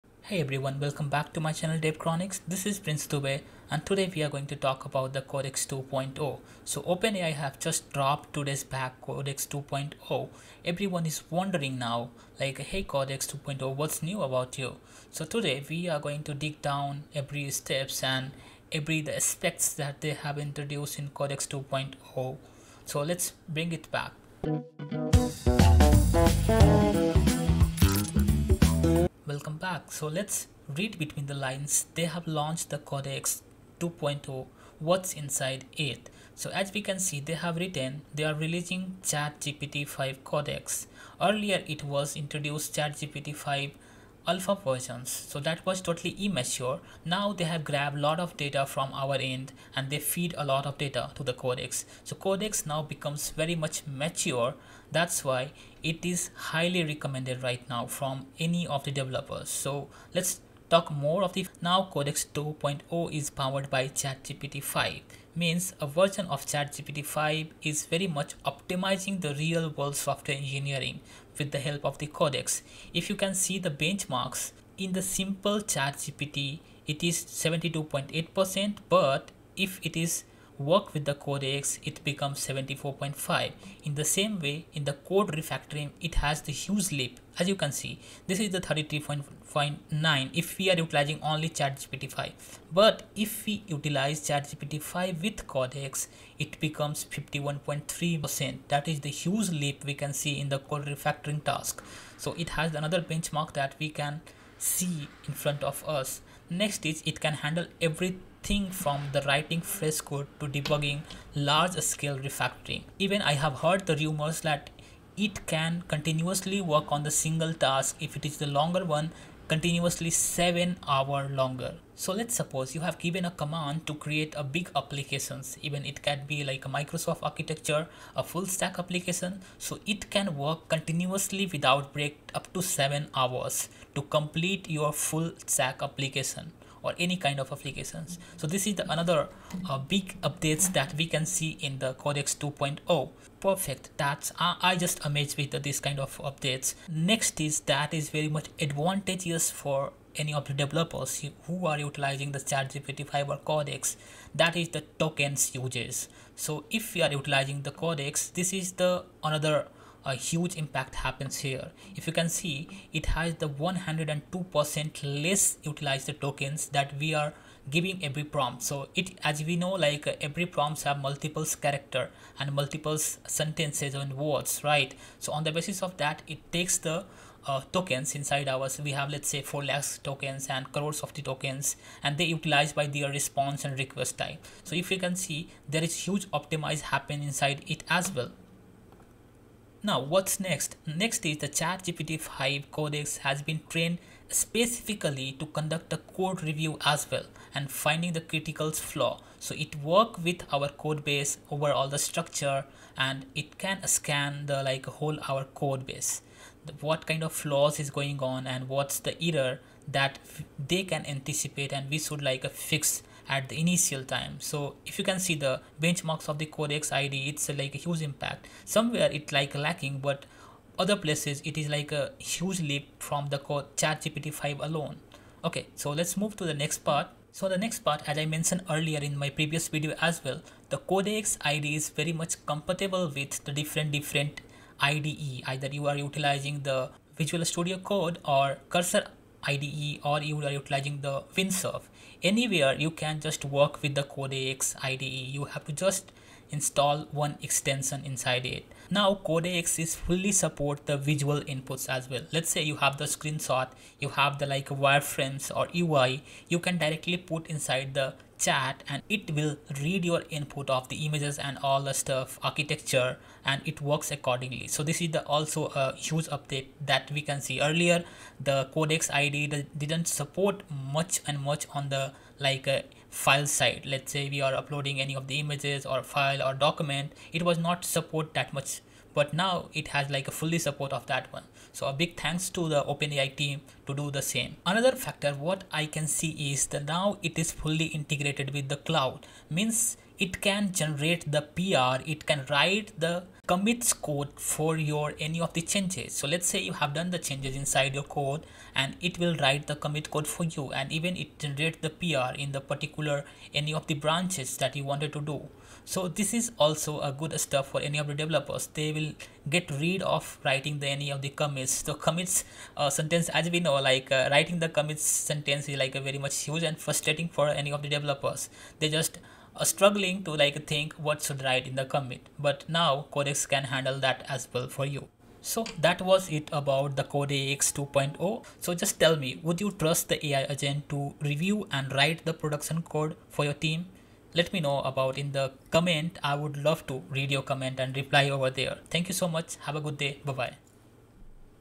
OpenAI Codex 2.0 Full Review 🔥: Codex upgrade Solves Cursor’s 'free token' & 'trial account' Problem
Welcome to Dev Chronics! I'm Prince Dubey, let's begain...

Introducing upgrades to Codex!
OpenAI just launched the brand-new **Codex IDE Extension**, powered by GPT-5 — and it’s a total game-changer for developers. 🚀

In this video, we’ll break down everything you need to know:
✅ Codex inside VS Code, Cursor & Windsurf
✅ Seamless local ↔ cloud handoff
✅ Built-in GitHub PR reviews with @codex
✅ GPT-5 power for smarter code generation & debugging
✅ Platform support (macOS, Linux, WSL for Windows)
✅ Live demo of Codex IDE in action

Whether you’re coding in JavaScript, Python, or building production apps, Codex is now your unified AI agent across IDE, CLI, GitHub, cloud, and even mobile.

💡 Why it matters:
- Boost developer **productivity**
- Eliminate **context switching**
- Improve **code quality** with intelligent AI reviews

👉 Watch till the end for a full walkthrough + demo!

🔹 **Chapters:**
0:00 Intro – Codex 2.0 Launch
0:30 What’s New with Codex IDE Extension
1:20 Cloud ↔ Local Seamless Workflow
2:00 GitHub PR Reviews with Codex 🛠️
2:40 GPT-5 Codex Power & Unified Agent
3:30 Platform Support: macOS, Linux, WSL
3:50 Early Developer Reactions & Use Cases
4:30 Why This Update Matters for Developers 💡
5:10 Outro + Final Thoughts

⏱️ 0:00 – 0:30 | Intro Hook

⏱️ 0:30 – 1:20 | What is Codex 2.0?

Codex 2.0 = GPT-5 Codex optimized for coding
Not just for small snippets, but real-world engineering → refactoring, debugging, building projects from scratch
Works everywhere: terminal, IDE (VS Code, Cursor), GitHub, even on your phone via ChatGPT app.
Integrated into ChatGPT Plus, Pro, Business, Edu, Enterprise plans.

⏱️ 1:20 – 2:20 | Speed & Reliability 🚀

Faster responses → dynamic reasoning → short tasks are super quick, big refactors handled in hours.
Example: Codex independently worked 7+ hours continuously fixing tests, iterating, and shipping code.
Efficiency upgrade: 93% fewer tokens on small tasks → snappier, lightweight.
Smarter handling of complex code → spends 2x more time thinking when task is heavy.

⏱️ 2:20 – 3:20 | New Features 🛠️

Codex CLI Revamp
Attach screenshots, wireframes, diagrams in CLI itself
Built-in to-do tracking, web search, MCP integration
Approval modes: read-only, auto, full access
Much cleaner terminal UI
Codex IDE Extension
Works inside VS Code, Cursor, Windsurf
Uses local context (open files, selections) → shorter prompts, better results
Seamless cloud ↔ local task handoff

⏱️ 3:20 – 4:20 | Cloud & Code Review ☁️

Codex Cloud Agent

90% faster task completion (container caching)
Auto environment setup, dependency install (pip install, etc.)
Can spin up browser, check frontend UI, send screenshots
Code Review Upgrade
Trained to catch critical bugs
Reviews entire codebase + dependencies
Can be invoked in GitHub with @codex review
Focuses on high-impact comments (less noise, more signal)

⏱️ 4:20 – 5:20 | Real-World Usage 🌍

Companies using Codex: Cisco Meraki, Duolingo, Virgin Atlantic, Ramp, Vanta, Gap
Example: Cisco used it to refactor + generate tests for a big feature release → on time delivery, no extra risk.
Works equally well on frontend tasks (desktop + mobile UI improvements).
Human preference studies → developers consistently rated Codex output higher.

⏱️ 5:20 – 6:00 | Pricing, Safety & Closing ⚖️
Included in ChatGPT Plus, Pro, Business, Edu, Enterprise
Sandbox environment → network access off by default (safe by design)
Approval required for risky commands
Reviews always recommended alongside human reviewers

What do you think—will Codex replace junior devs, or will it make all of us 10x engineers? Drop your thoughts in comments.